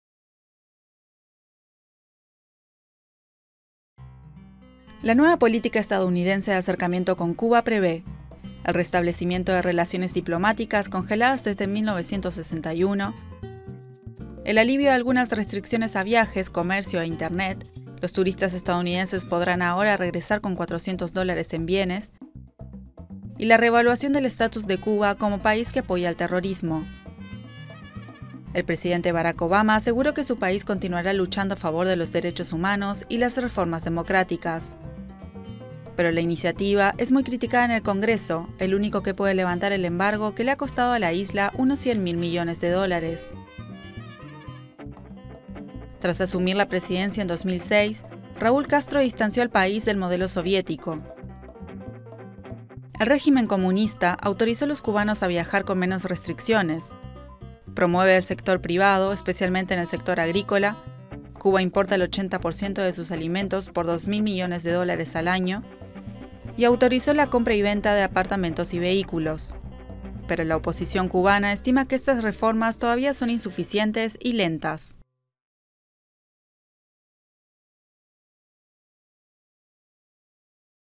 El acercamiento EEUU-Cuba
El presidente de Estados Unidos, Barack Obama, y su par cubano Raúl Castro dieron un paso histórico hacia la normalización de sus relaciones bilaterales, poniendo fin a más de medio siglo de enfrentamientos. Videografía sobre el acercamiento EEUU-Cuba.